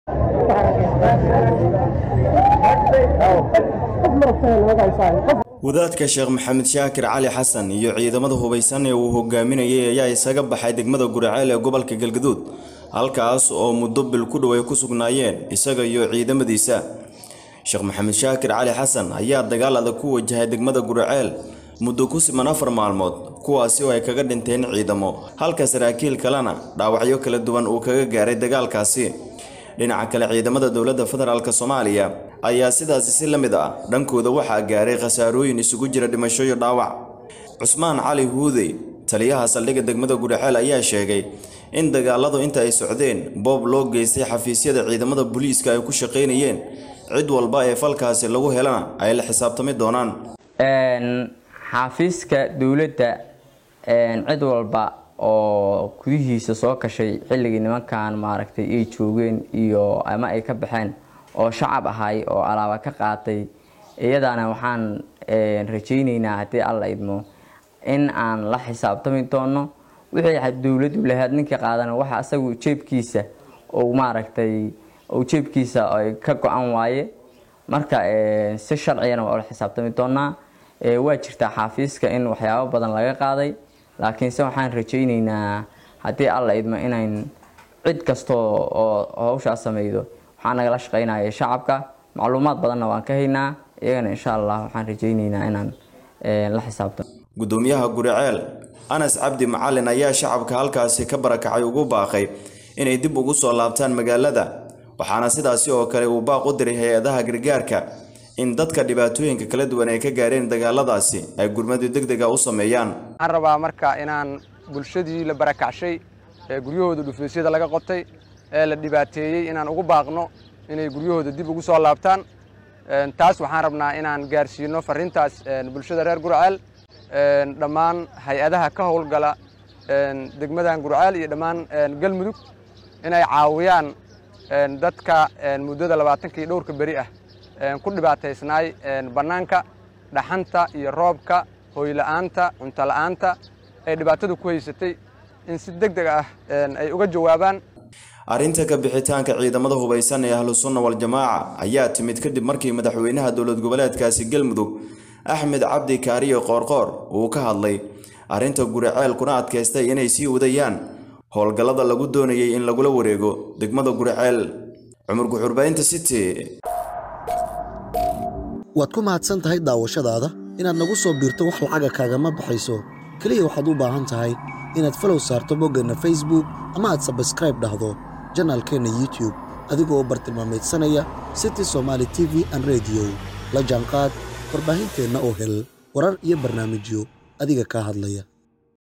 Taliska Booliiska Guriceel oo Sheegay in la boobay Xafiiska ay Ku shaqeynayeen Booliiska
Taliska Booliiska Guriceel oo Sheegay in la boobay Xafiiska ay Ku shaqeynayeen Booliiska Cumar Guux ayaa kawaramaya